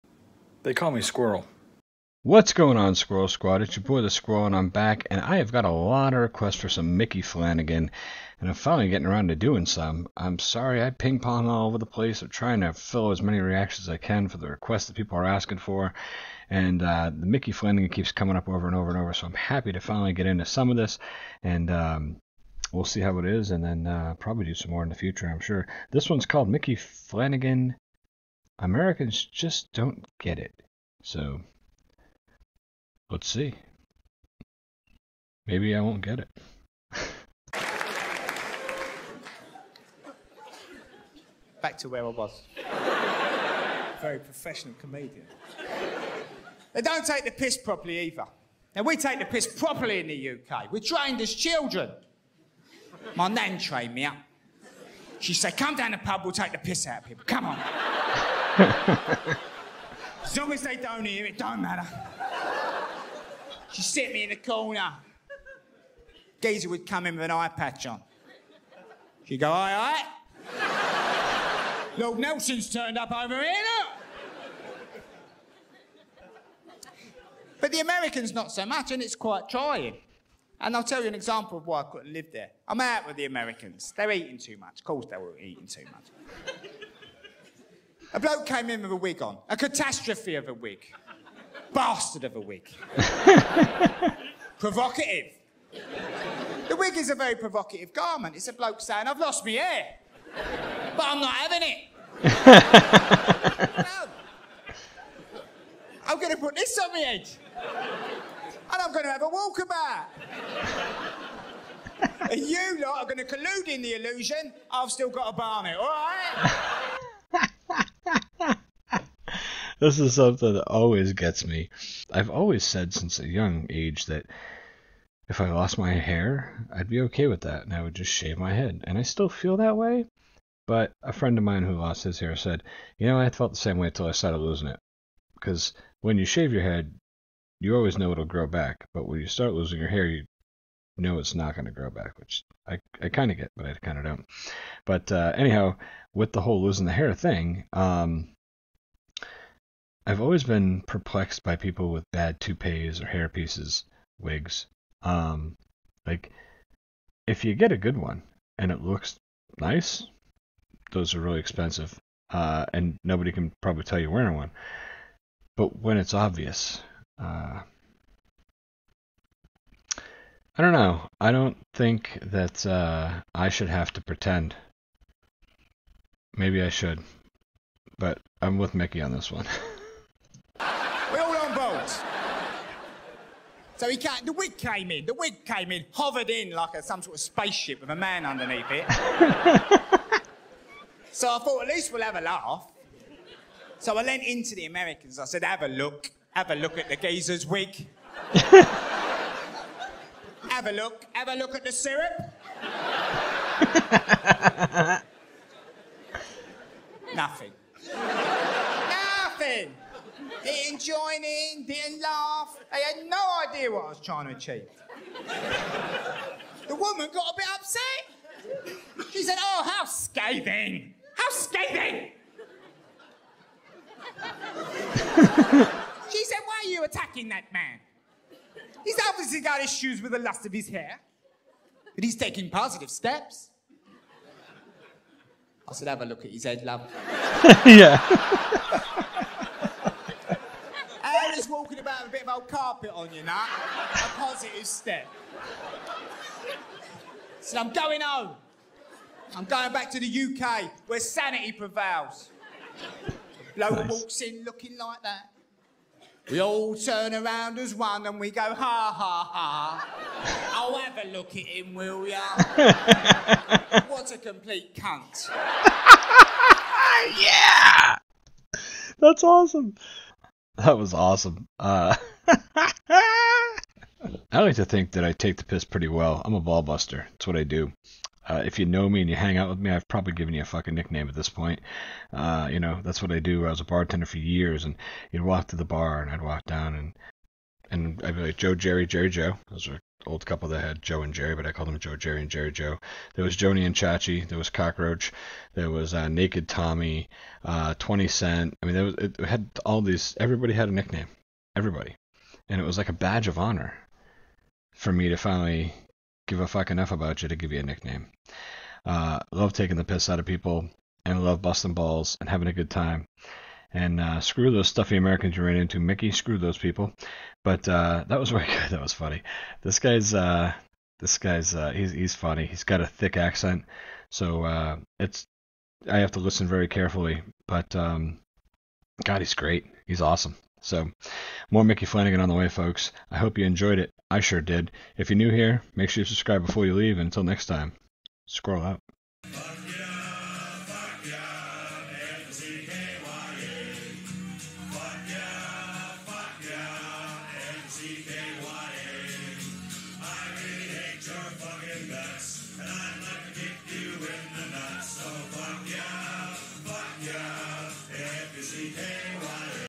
American Reacts To Micky Flanagan Americans just don't get it | Comedy Reaction | Squirrel Reacts
This is funny as hell and I will be doing more very soon.

Send me something:
01506